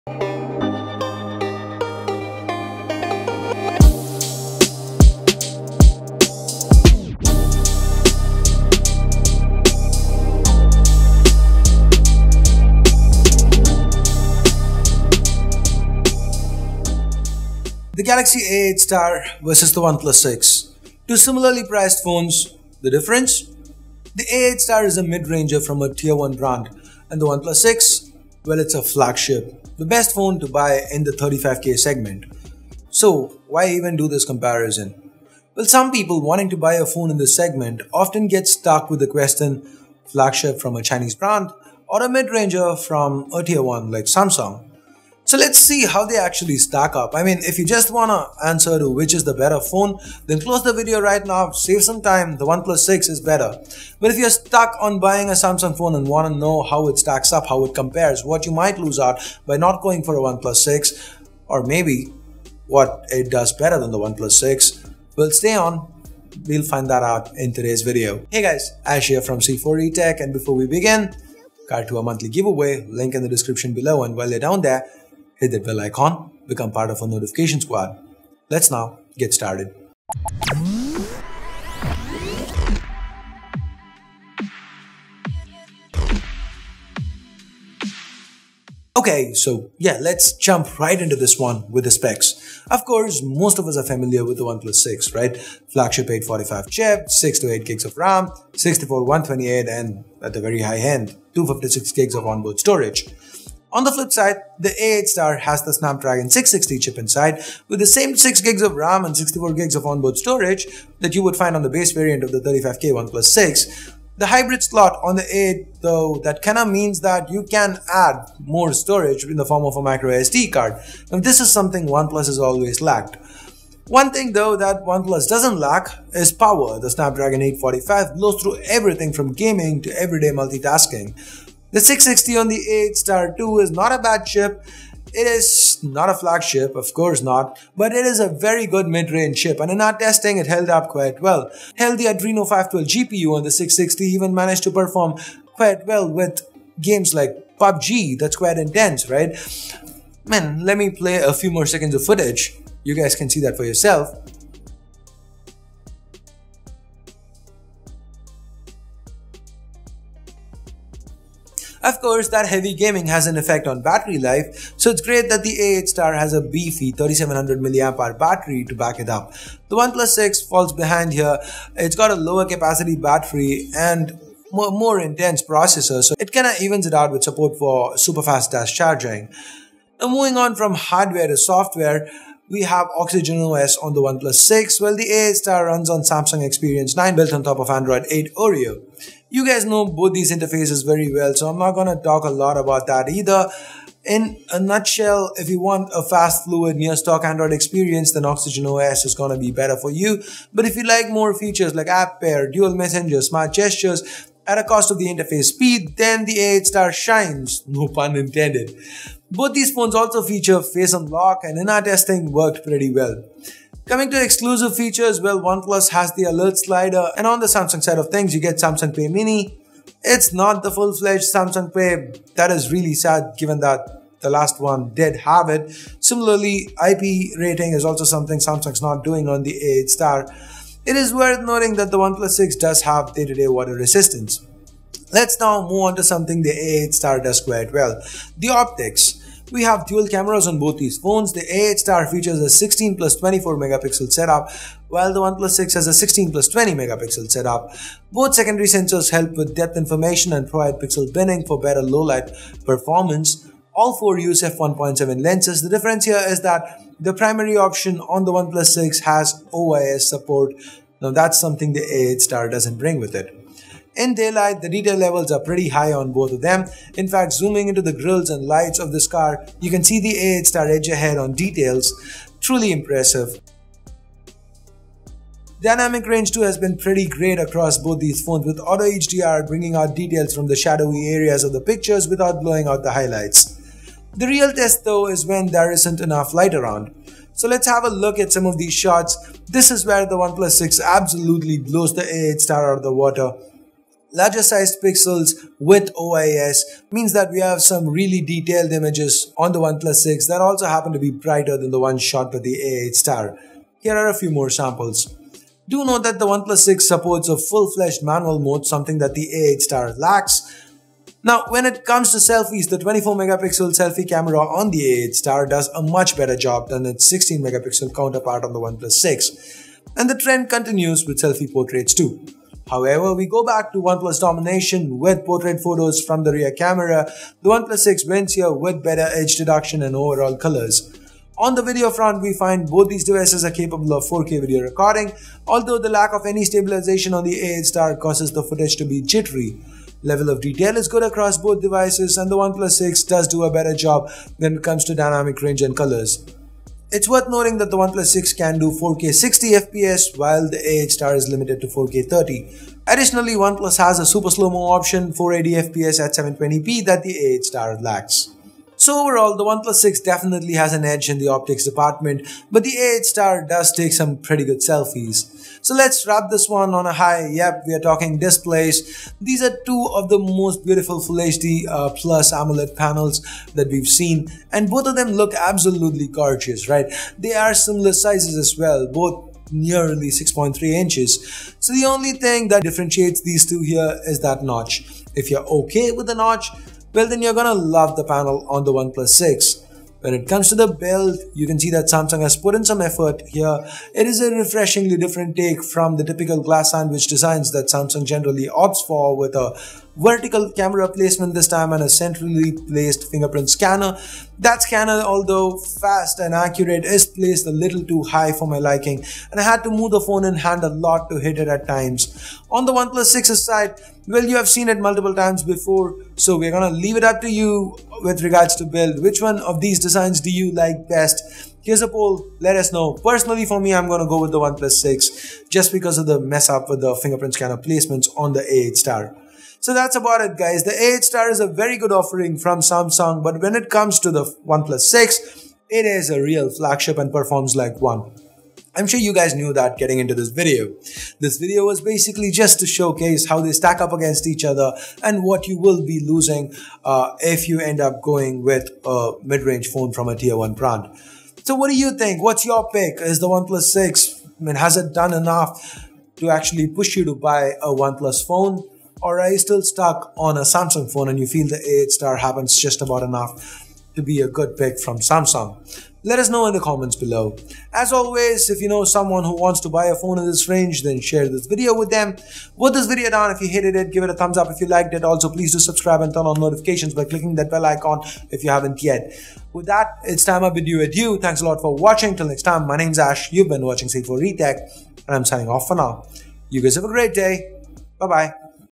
Chinese Flagship KILLS Samsung at Same Price??? HONEST Comparison!
Samsung recently launched their upper midrange Galaxy A8 Star. Since the A8 Star has similar pricing as the OnePlus 6, in this video, let's compare the Galaxy A8 Star with the OnePlus 6 in detail - see if Samsung's just overpriced and what all you lose if you choose Samsung over OnePlus.

Both the Samsung Galaxy A8 Star and the OnePlus 6 sport roughly the same size displays at around 6.3 inches - the resolutions are the same too Full HD Plus and so's the tech - Super AMOLED.

The OnePlus 6 does sport a notch thouhg.

Now while the Galaxy A8 Star runs on Qualcomm's midrange Snapdragon 660 octa core processor, the OnePlus 6 sports the flagship Snapdragon 845 - both have 6 GB RAM and 64 GB internal storage - the a8 star supports micro sd while the OnePlus 6 offers additional variants.

On the battery front, the OnePlus 6 has a 3300 mAh battery while the Galaxy A8 Star sports a larger 3700 mAh battery.

Both phones have dual camera setups - the A8 Star has a 16+24 MP setup while the OnePlus 6 has a 16+20 MP setup all cameras are paired with f/1.7 lens. The OnePlus 6 sports OIS on it's primary camera though

The Galaxy A8 Star runs on Samsung Experience 9 built on top of Android 8 Oreo.

If you liked our "Chinese Flagship KILLS Samsung at Same Price??? HONEST Comparison! Comments are welcome too :)